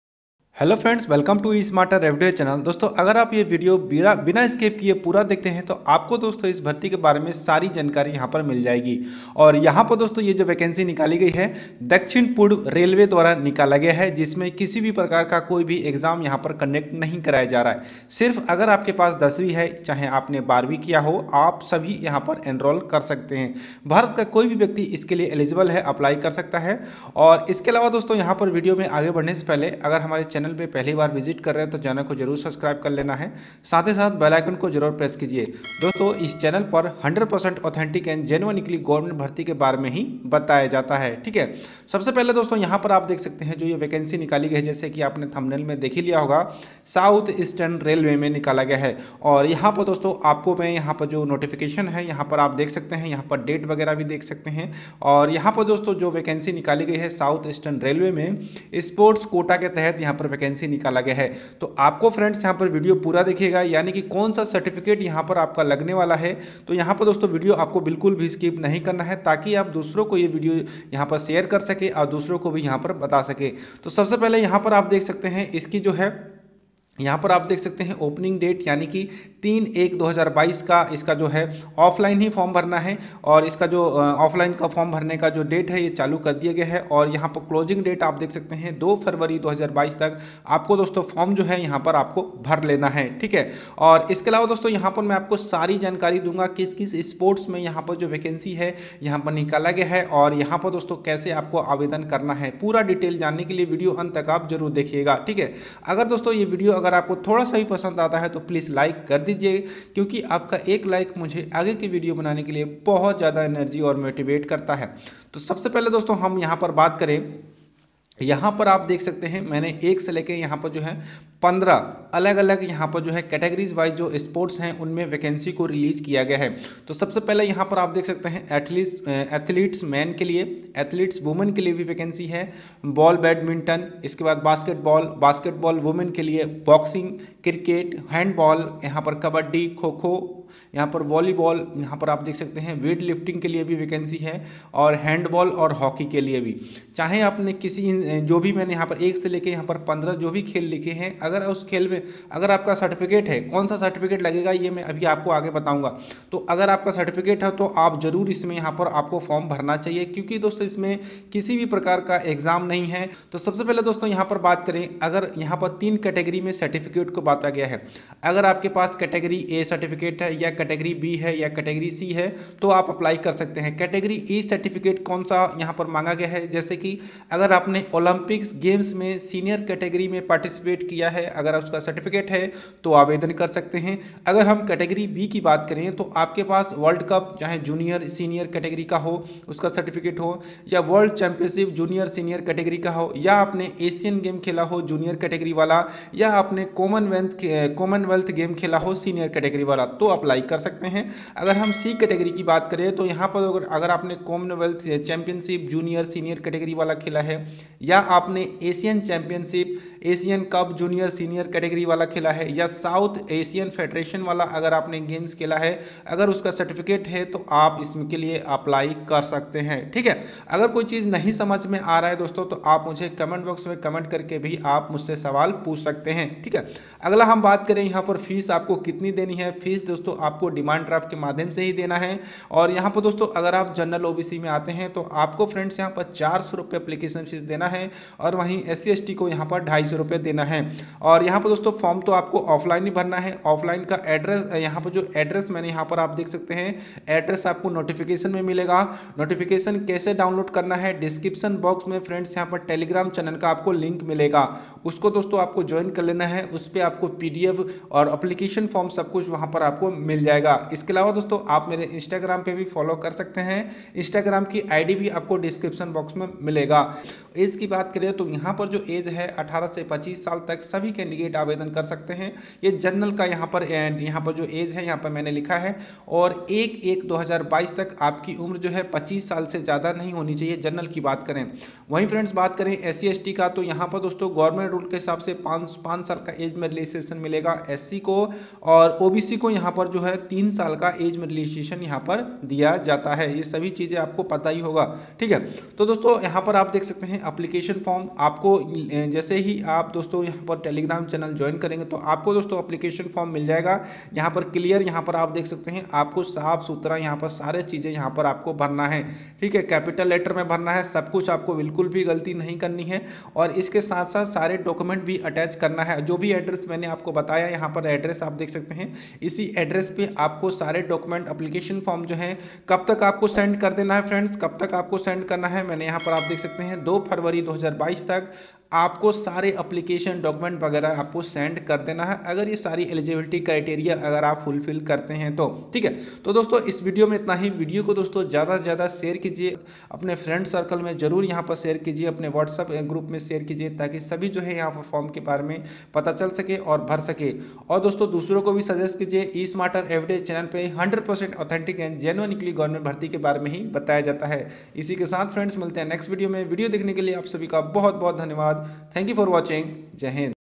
Railway Recruitment 2021 | SER Sports Quota 2022 | No Exam | कौन सा Certificate चाहिए || 10th/12th
South Eastern Railway is inviting online applications from eligible candidates for filling up various vacancies for the post of Sports Quota. Applicants who are interested can apply for South Eastern Railway Sports Quota jobs by sending the SER application form before the last date.

Note:-आवेदन भरने के लिए  SER Sports Quota   द्वारा जारी किया गया नोटिफिकेशन को ध्यान पूर्वक पढ़ने के बाद ही आवेदन करें |